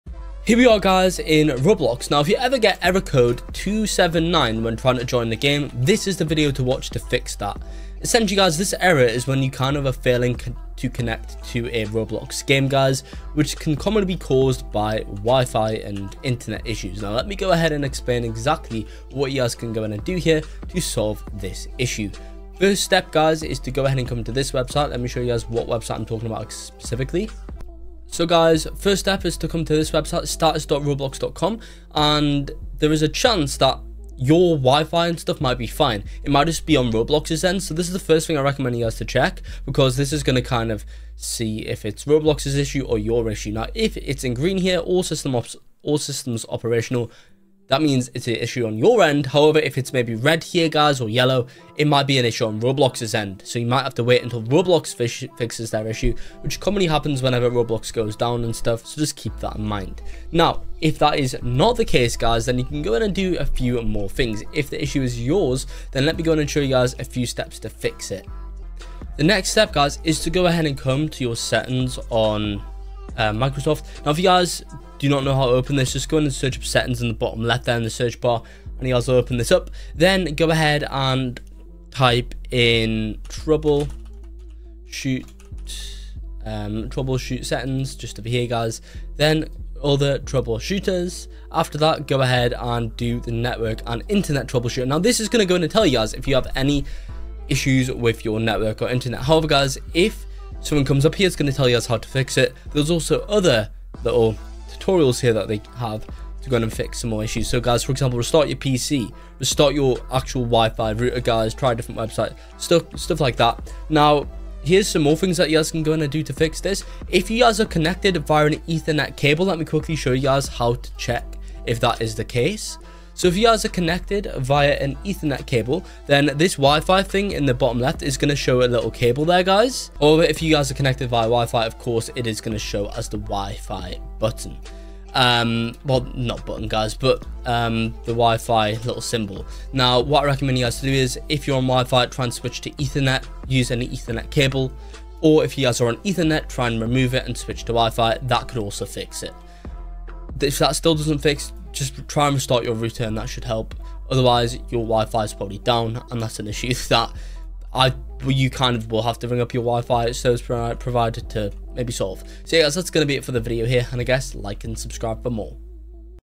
How To FIX ROBLOX ERROR CODE 279! (Roblox Failed To Connect To Game ID 17)
This video shows you how to fix the roblox error code 279. This error code is when you cant connect to a roblox game. It sometimes shows roblox game id 17.